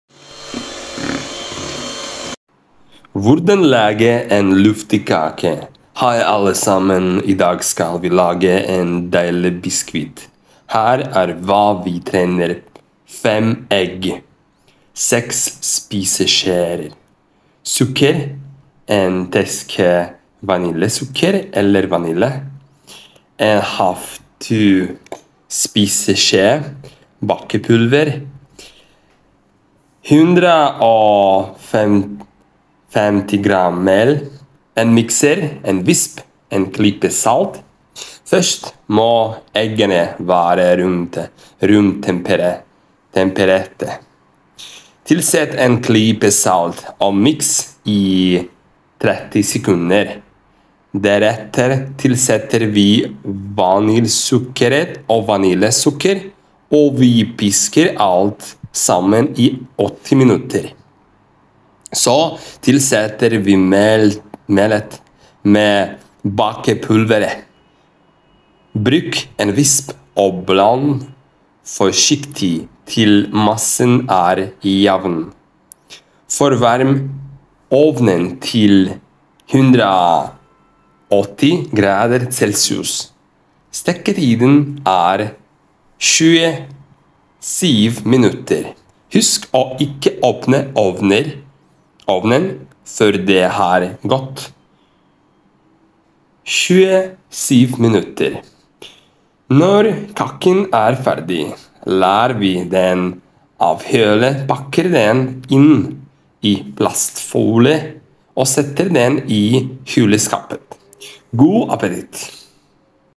Hvordan lage en luftig kake
Hei alle sammen! I dag skal vi lage en deilig biskvit. Her er hva vi trenger:

• 5 egg

• 6 spiseskjeer sukker

• 1 teskje vaniljesukker eller vanilje

• ½ spiseskje bakepulver

• 150 gram mel

• En mikser

• En visp

• En klype salt

Først må eggene være romtempererte. Tilsett en klype salt og miks i 30 sekunder.

Deretter tilsetter vi vaniljesukkeret og vanlig sukker, og vi pisker alt sammen i 8 minutter.

Så tilsetter vi melet med bakepulveret. Bruk en visp og bland forsiktig til massen er jevn.

Forvarm ovnen til 180 grader Celsius. Steketiden er 27 minutter.

Husk å ikke åpne ovnen før det har gått 27 minutter!

Når kaken er ferdig, lar vi den avkjøle, pakker den inn i plastfolie og setter den i kjøleskapet.

God appetitt!